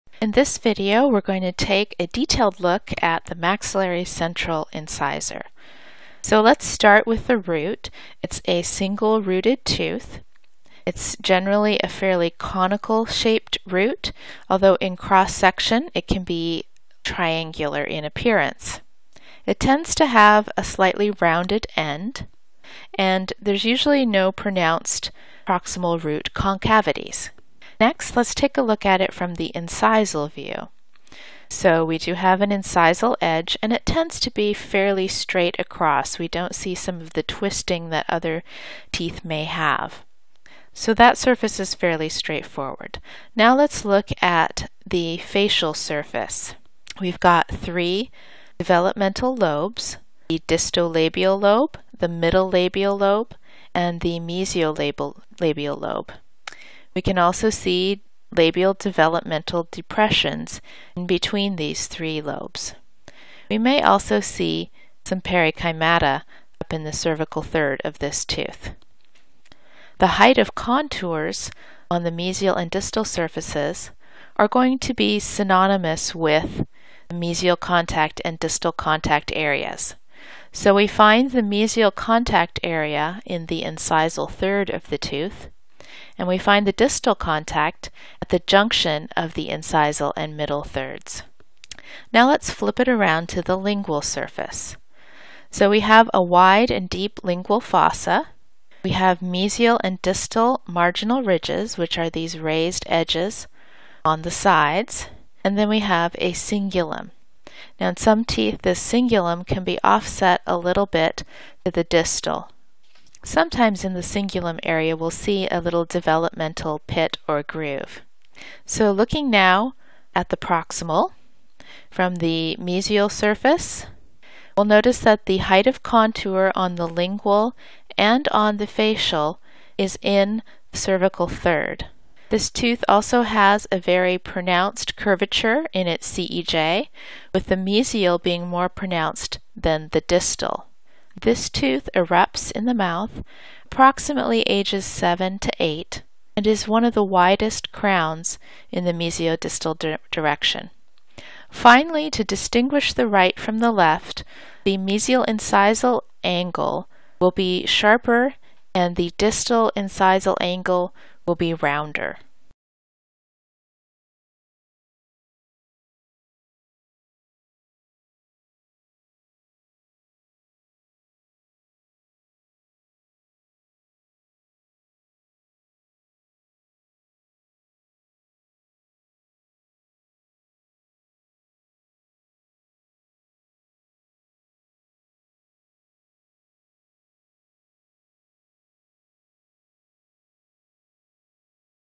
Maxillary Central Incisor Dental Anatomy
A tour of the anatomical features of the maxillary central incisor, including coronal and root anatomy.  This is an updated video from my previous incisor video.